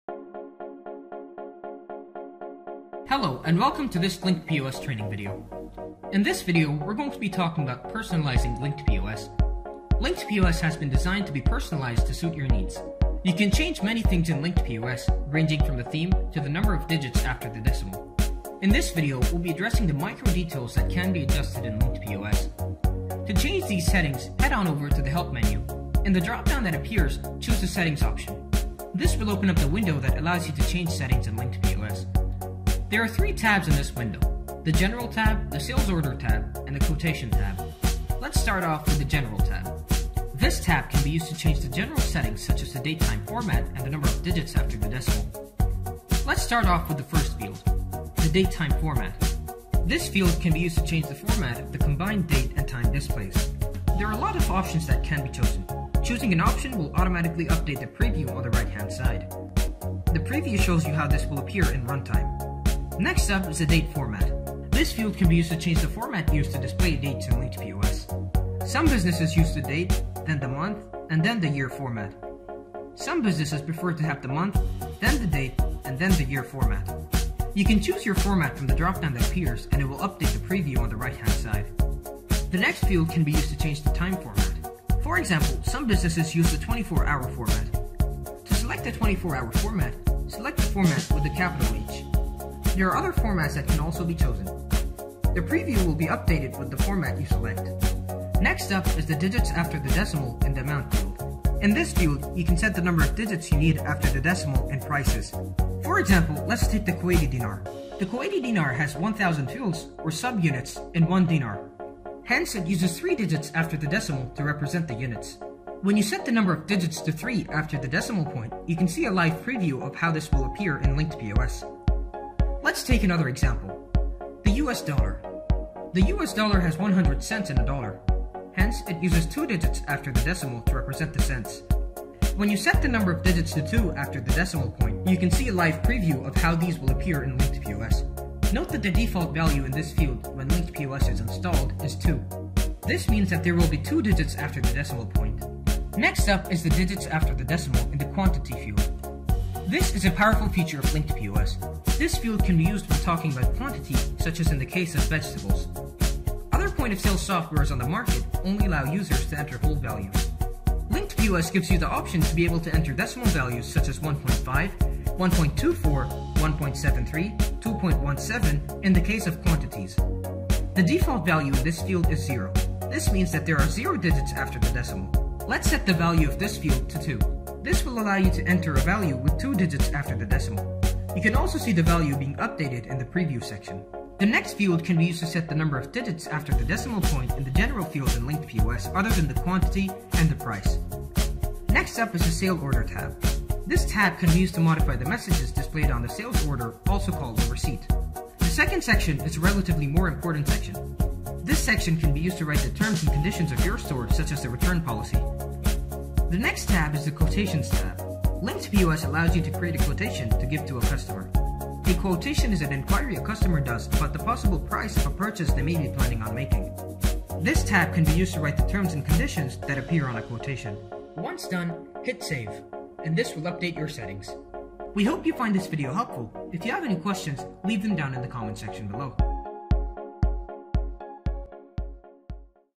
Changing Amount, Quantity & Date-Time Display Formats | LinkedPOS
Not every retail business sells items in whole digit quantities. Some of the businesses sell in fractional quantities. Similarly, different currencies require a different number of digits after the decimal. LinkedPOS has an in-built feature to allow the customization of display of different numerical figures like amount & quantity as well as date & time. This video is a step by step guide for customization.